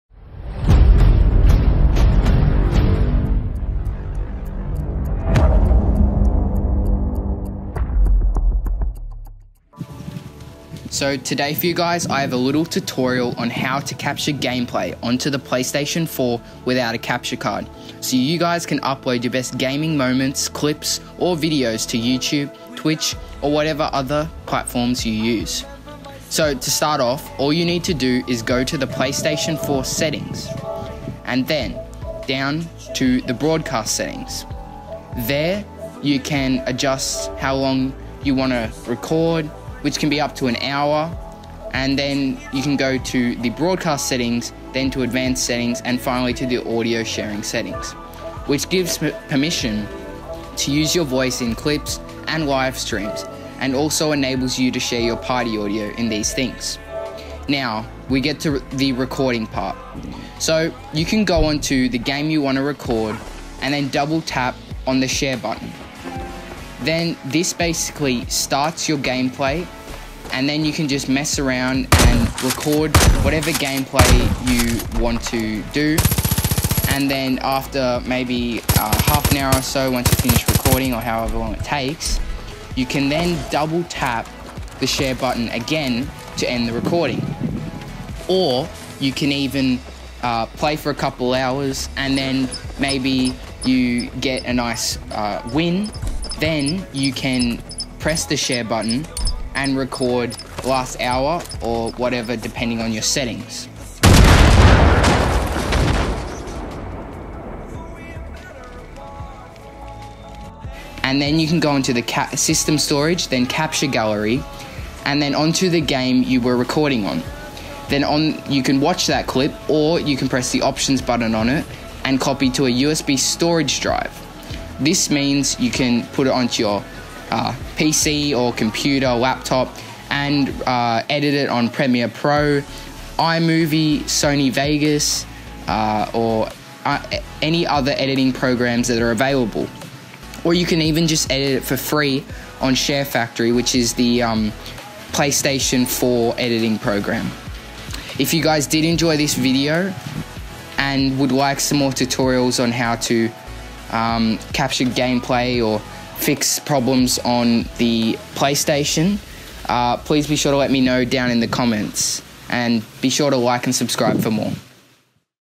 How to Capture Gameplay on the PS4!! - (No Capture Card)
Hey Guys Choppahawk here and today for you guys I have a tutorial video on how to capture gameplay on the PS4.